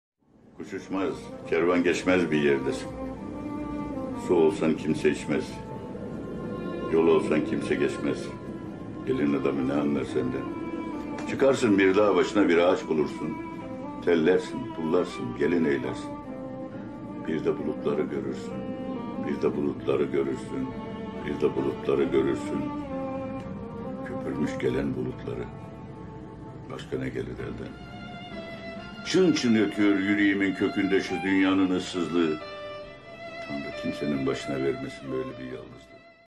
Yaşar Kemal: Yol olsan kimse geçmez, su olsan kimse içmez
Yaşar Kemal (Kemal Sadık Gökçeli), romancı, senaryo ve öykü yazarı. (6 Ekim 1923, Hemite
- 28 Şubat 2015, İstanbul)